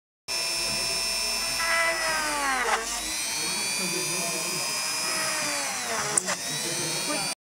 Wind chime - holes part 04
Wind chime - planning and making holes for assembly and hanging